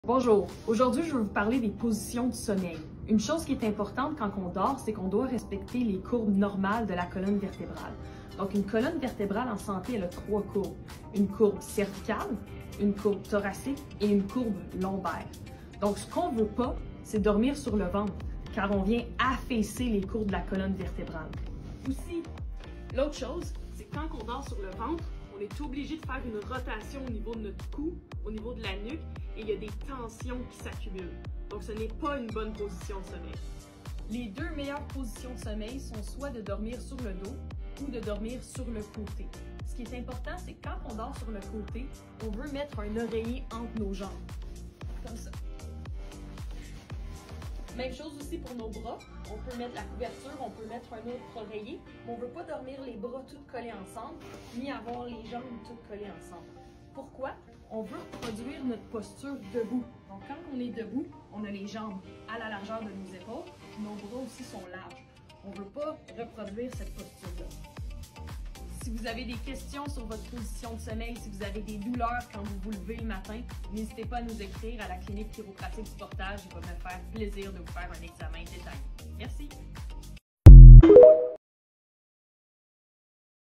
Ne pas dormir sur le ventre - Quelles sont les meilleures positions de sommeil?
Vous voulez éviter les douleurs au bas du dos ou au cou durant la nuit? Voici les conseils de Dre Émilie Gaudreau, chiropraticienne pour une meilleure position de sommeil.

N'oubliez pas, évitez de dormir sur le ventre!!!

Pour plus d'informations, contactez-nous!